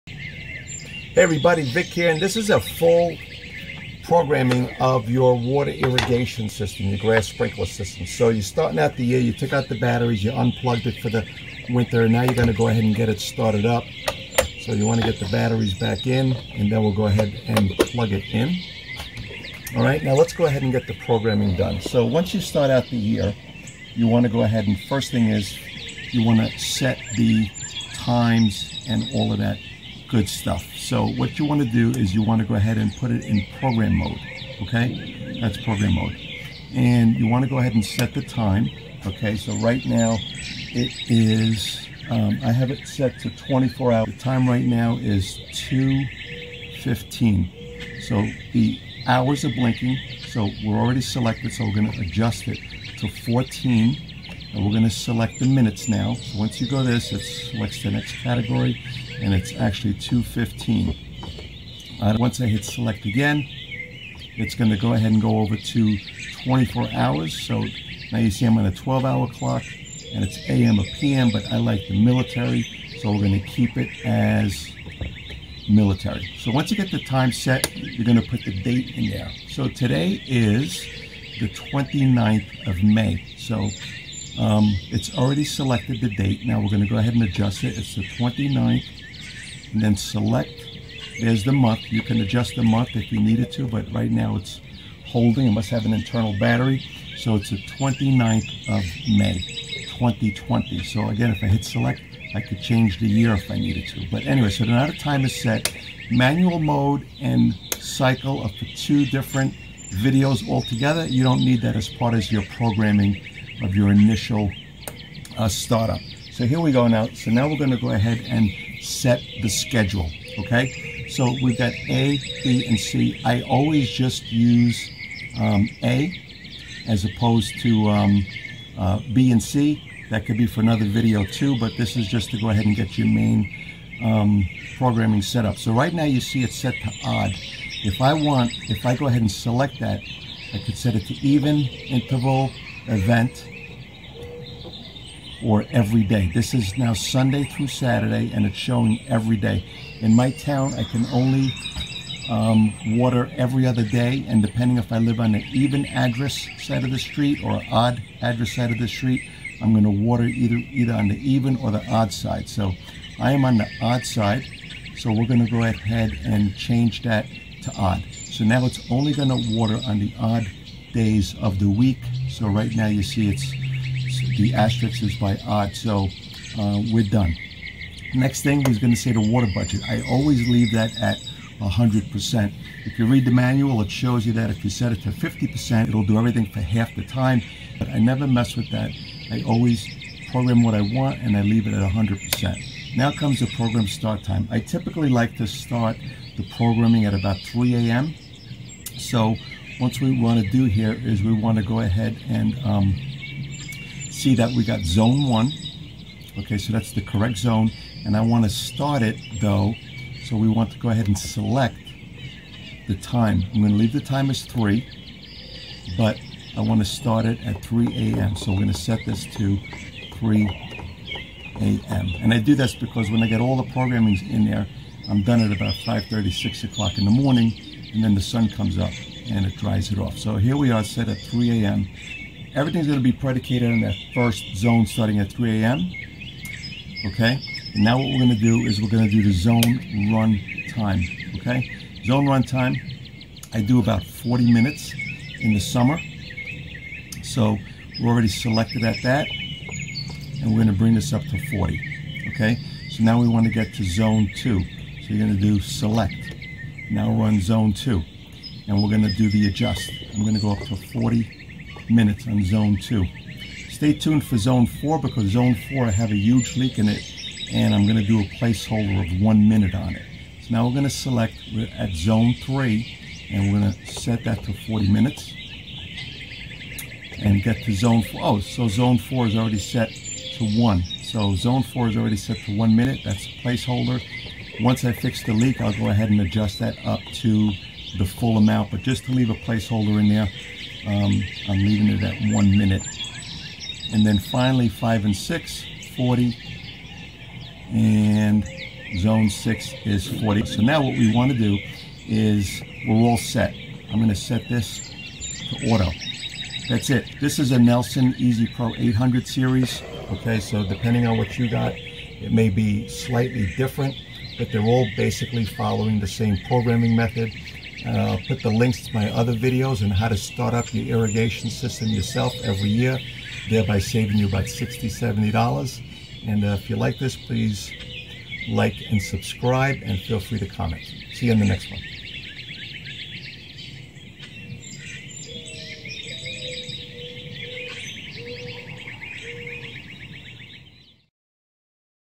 How To Program Grass Sprinkler Automatic System DIY Complete Tutorial
Simple Do It Yourself on How To Program Your Automatic Grass Sprinkler System Quick Start DIY Tutorial
Then check out my video on starting up your automatic underground grass sprinkler system for spring. Do it yourself with these instructions and save $60 to $75 or more
Check out these state of the art Irrigation controllers if you're looking to update yours:
Orbit 12-Zone Smart Controller - Control from your Mobile Phone
Rachio 8 Zone Smart Controller - Control from your Mobile Phone

This video will walk you through simple step by step process to program your Automatic Grass Sprinkler system